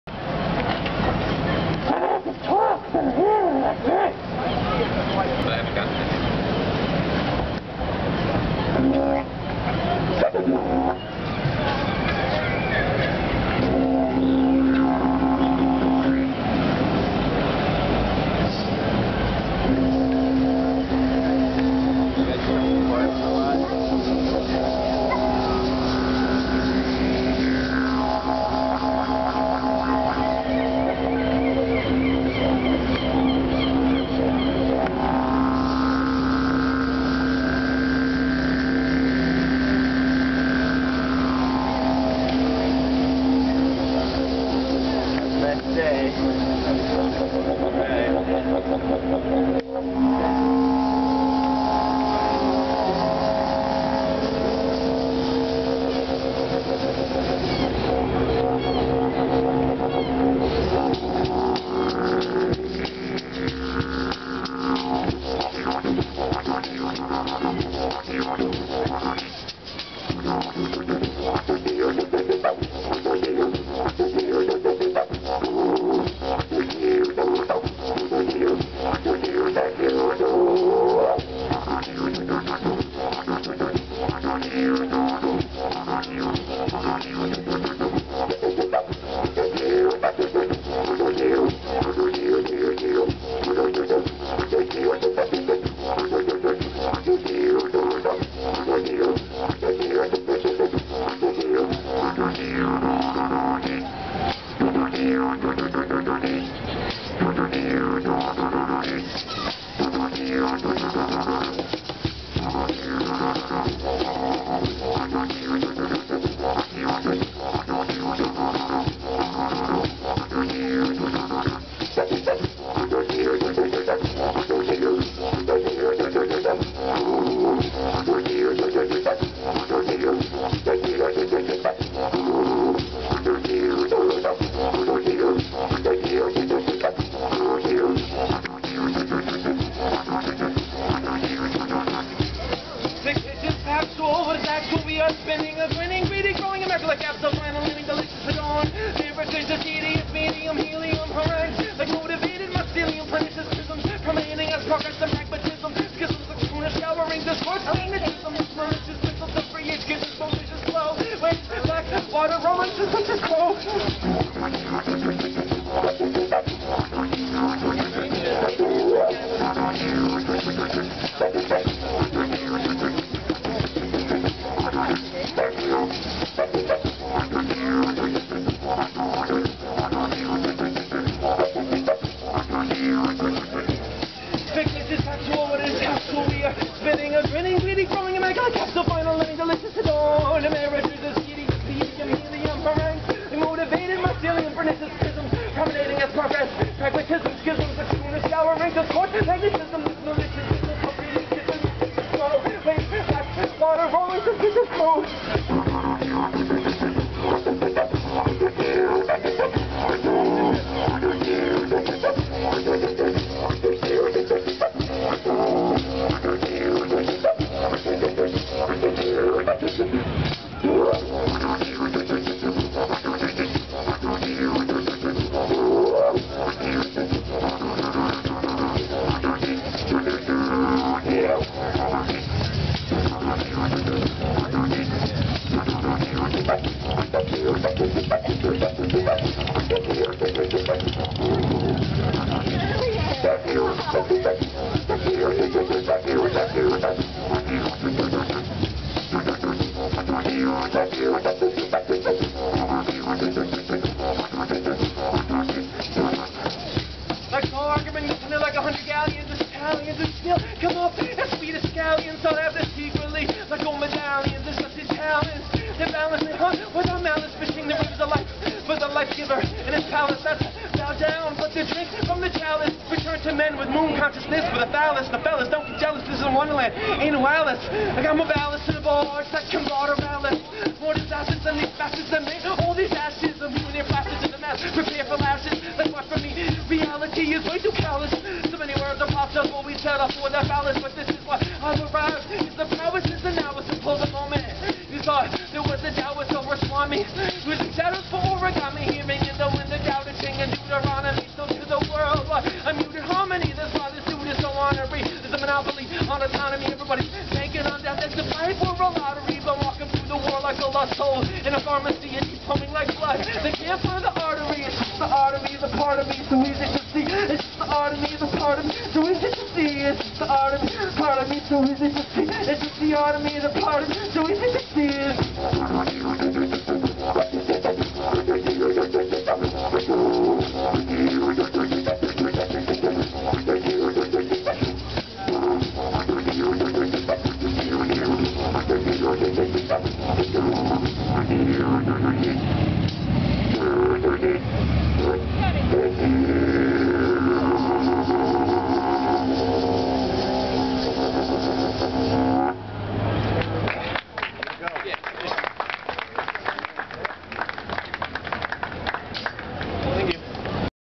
Tree 1
Michael Sampson's crazy busking routine, Portland, ME